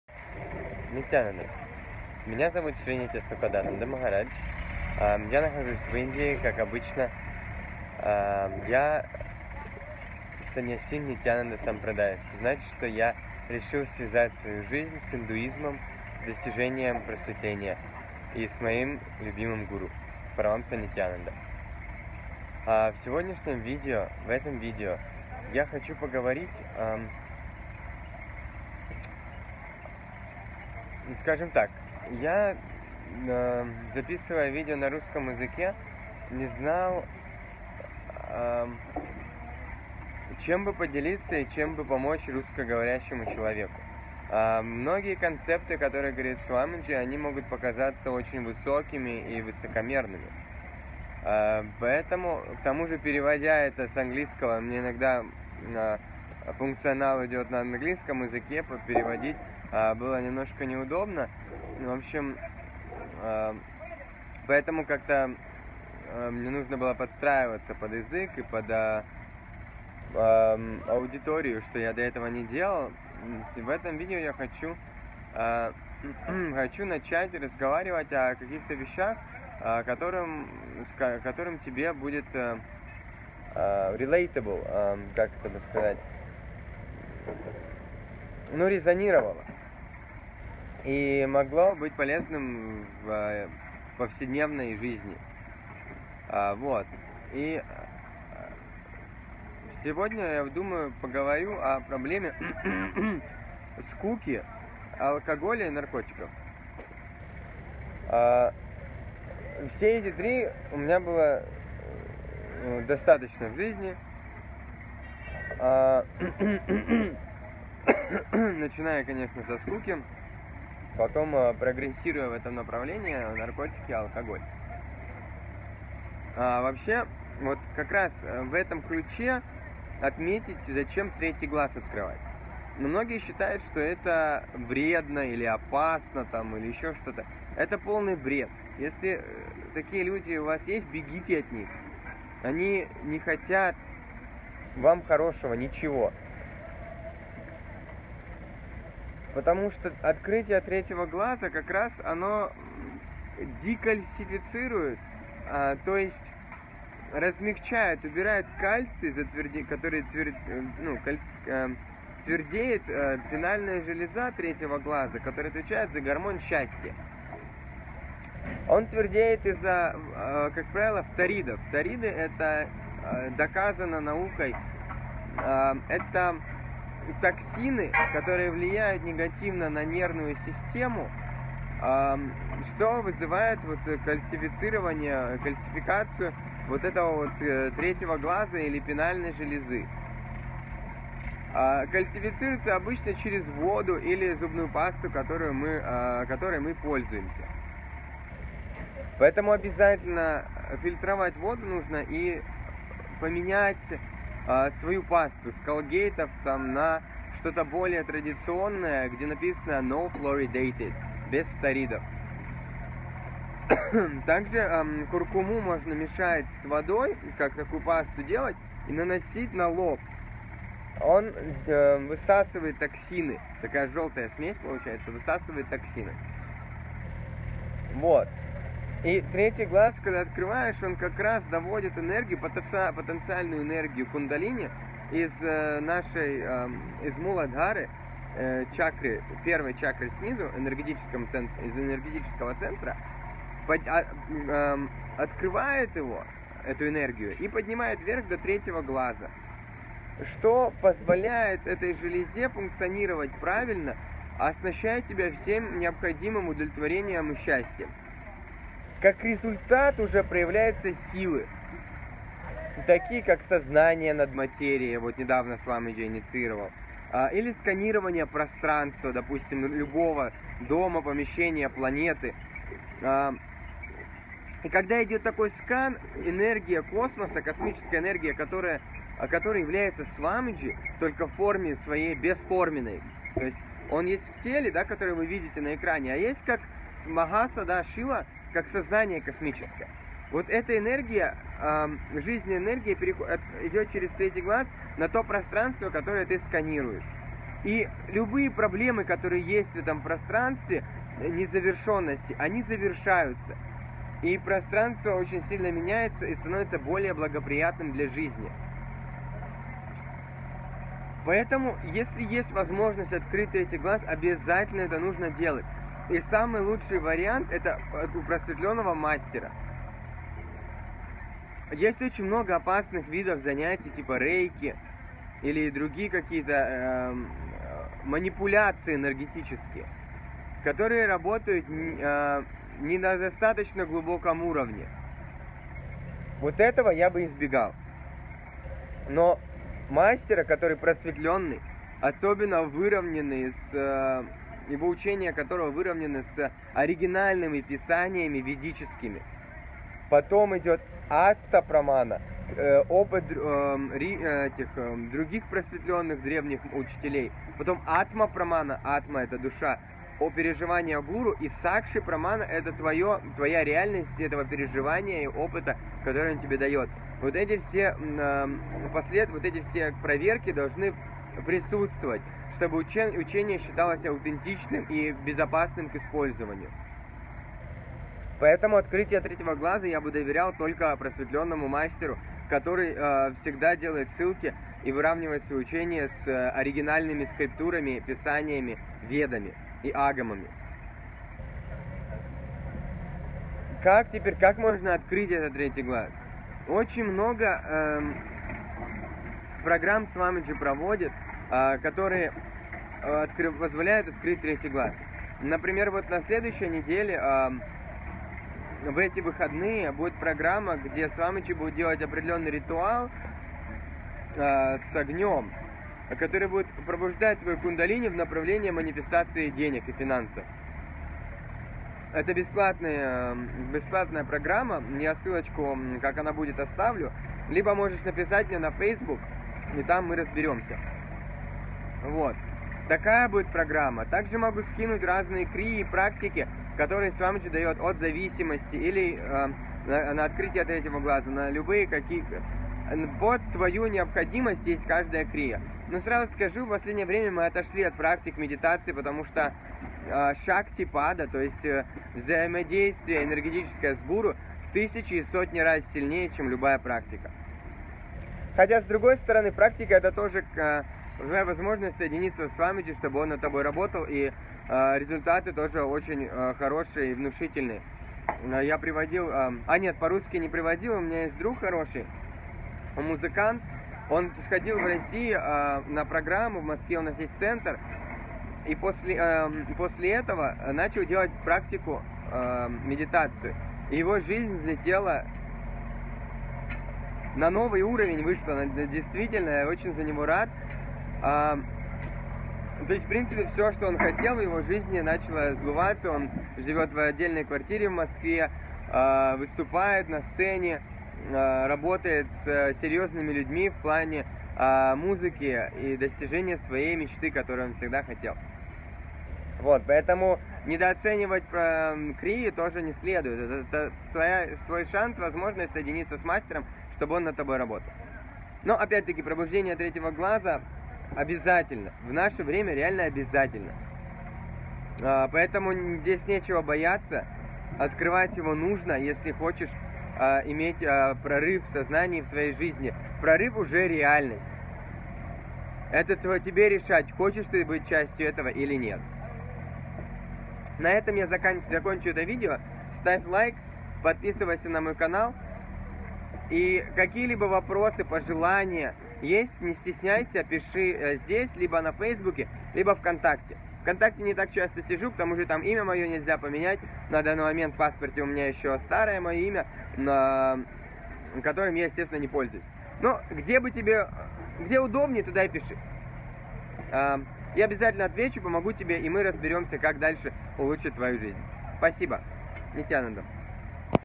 Жизнь без наркотиков! Как этого добиться?
Чувство любви Мастера - это самое нежное, мотивирующее и удивительное чувство на свете. Ни один наркотик не способен тебе заменить секунду, проведенную с Ним. Открытие третьего глаза просто необходимо каждому, чтобы испытать радость и удовлетворение. Пинеальная железа как раз-таки выделяет гормон, ответственный за счастье человека. Эта железа блокирована токсинами, которые добавляют в зубную пасту и воду, чтобы ты оставался послушным и несчастным.
Открытие третьего глаза - это твоя собственная галактика, которая позволяет тебе исполнять свои желания и желания других людей.

Вместо манипуляций энергиями через магию и другие небезопасные техники, лучше доверь открытие своего потенциала культуре, которая тысячелетиями и гораздо больше, доказала свою аутентичность и надежность - Ведической и Агамической традиции Санатаны Хинду Дхармы ( Индуизму).

Благословляю!